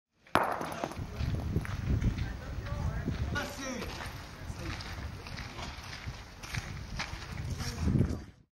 21 septembre 2024 à Mèze.
Concours inter-club. Boule d'Azur vs Fer Roulant